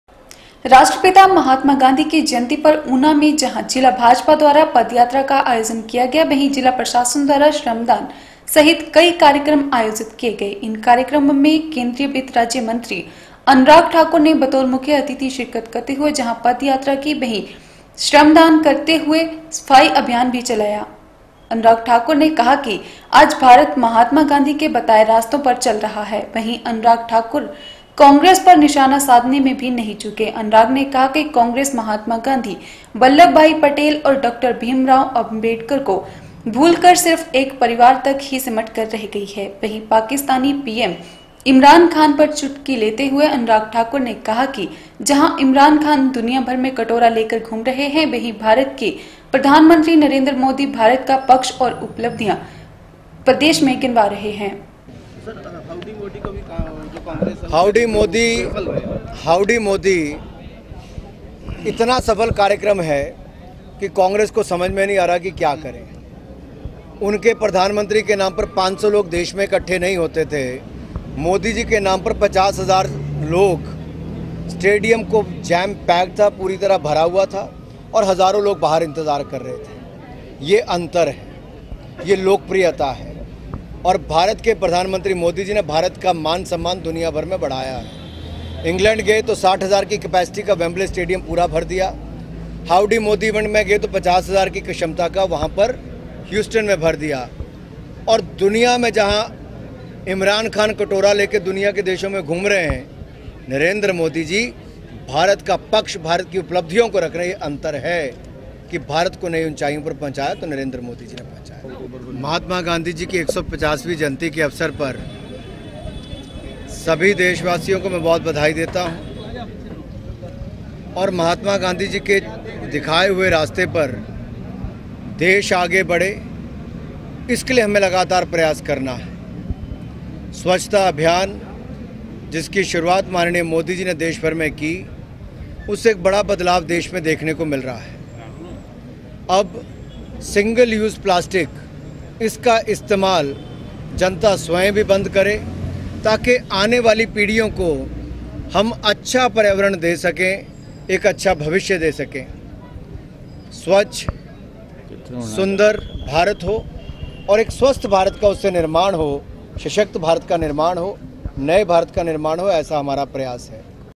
2 OCT N 2 In Una, where the march was organized by the district BJP
राष्ट्रपिता महात्मा गांधी की जयंती पर ऊना में जहाँ जिला भाजपा द्वारा पदयात्रा का आयोजन किया गया वहीँ जिला प्रशासन द्वारा श्रमदान सहित कई कार्यक्रम आयोजित किये गए। इन कार्यक्रमों में केंद्रीय वित्त राज्य मंत्री अनुराग ठाकुर ने बतौर मुख्यातिथि शिरकत करते हुए जहाँ पदयात्रा की वहीँ श्रमदान करते हुए सफाई अभियान भी चलाया।